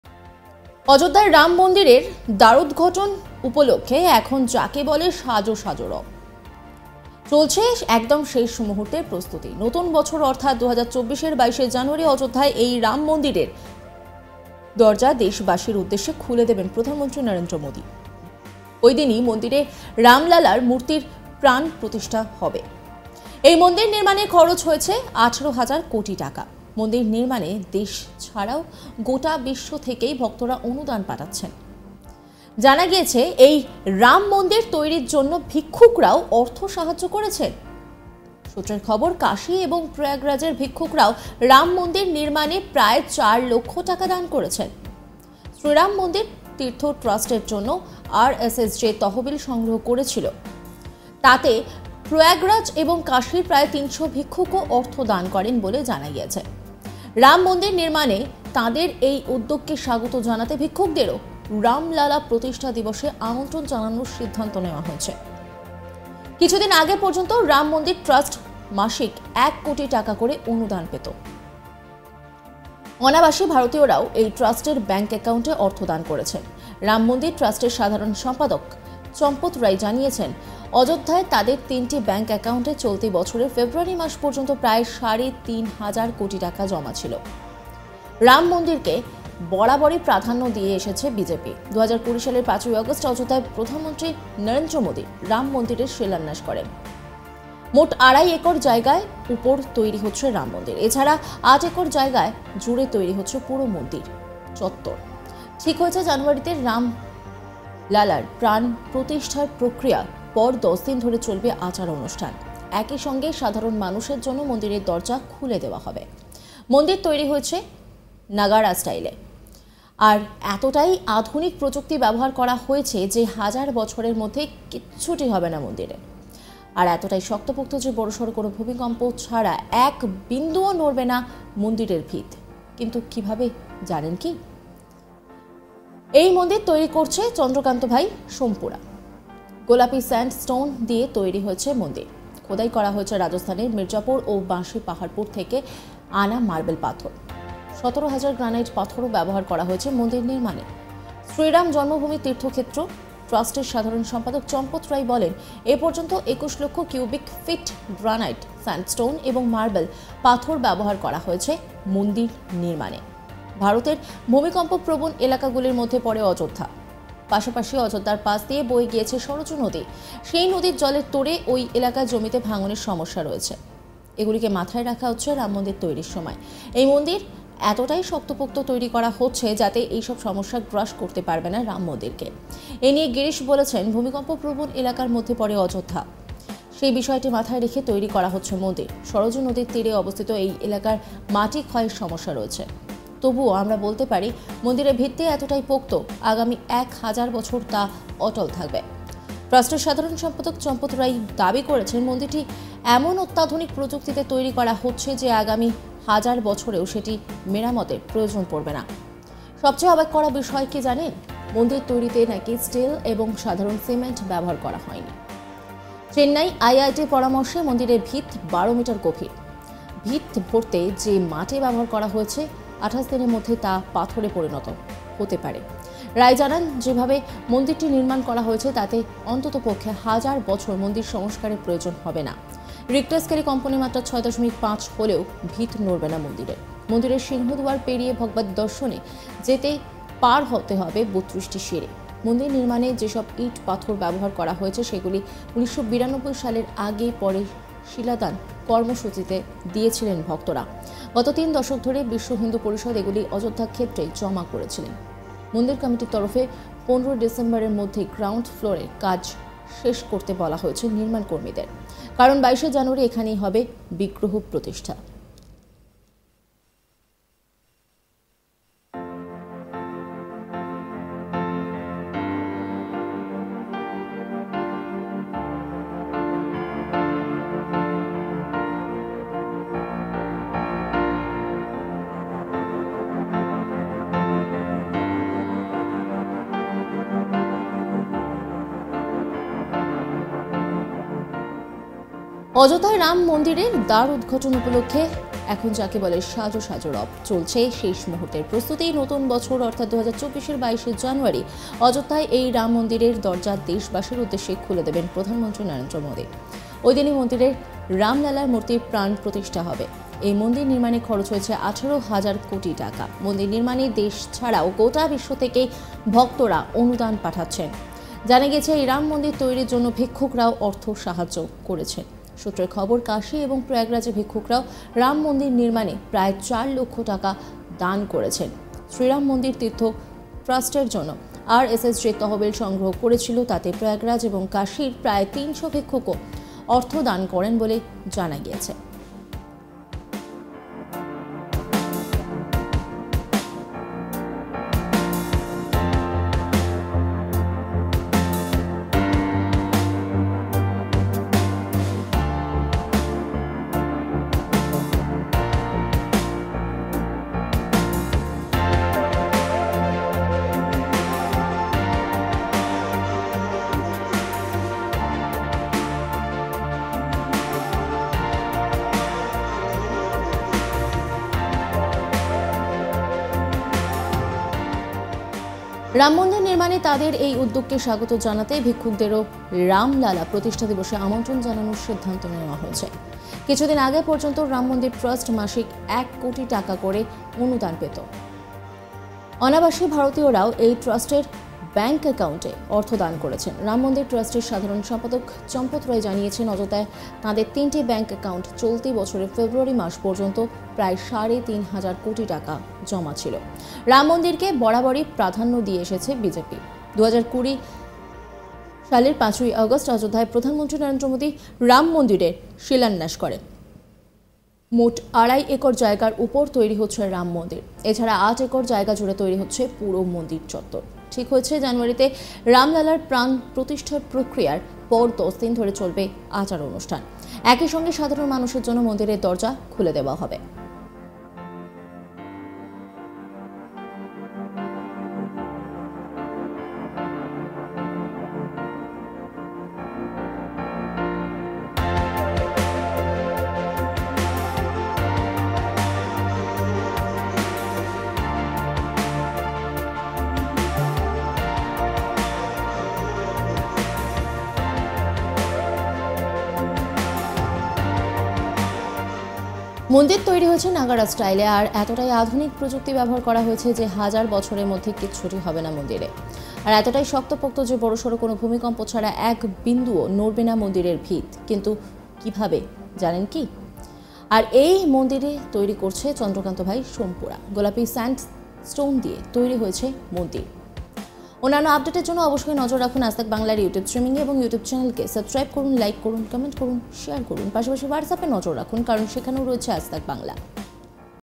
Breaking : রাম মন্দির তৈরিতে হিন্দু গরিব ভিক্ষুকরা সাড়ে 4 লক্ষ টাকা দিয়েছেন| Ayodhya Ram Mandir
অযোধ্যায় রামমন্দিরের দ্বারোদঘাটন উপলক্ষে এখন যাকে বলে সাজসাজ রব। চটলছে একদম শেষ মুহূর্তের প্রস্তুতি। নতুন বছর অর্থাৎ ২০২৪-এর ২২ জানুয়ারি অযোধ্যায় এই রামমন্দিরের দরজা দেশবাসীর উদ্দেশে খুলে দেবেন প্রধানমন্ত্রী নরেন্দ্র মোদি। ওই দিনই মন্দিরে রামলালার মূর্তির প্রাণ প্রতিষ্ঠা হবে। এই মন্দির নির্মাণের খরচ হয়েছে ১৮ হাজার কোটি টাকা। মন্দির নির্মাণে দেশ ছাড়াও গোটা বিশ্ব থেকেই ভক্তরা অনুদান পাঠাচ্ছেন। জানা গিয়েছে এই রাম মন্দির তৈরির জন্য ভিক্ষুকরাও অর্থ সাহায্য করেছেন। সূত্রের খবর, কাশি এবং প্রয়াগরাজের ভিক্ষুকরাও রাম মন্দির নির্মাণে প্রায় চার লক্ষ টাকা দান করেছেন। শ্রী রাম মন্দির তীর্থ ট্রাস্টের জন্য আরএসএস যে তহবিল সংগ্রহ করছিল, তাতে প্রয়াগরাজ এবং কাশির প্রায় ৩০০ ভিক্ষুকও অর্থ দান করেন বলে জানা গিয়েছে। রাম মন্দির নির্মাণে তাঁদের এই উদ্যোগকে স্বাগত জানাতে ভিক্ষুকদেরও রামলালা প্রতিষ্ঠা দিবসে আমন্ত্রণ জানানোর সিদ্ধান্ত নেওয়া হয়েছে।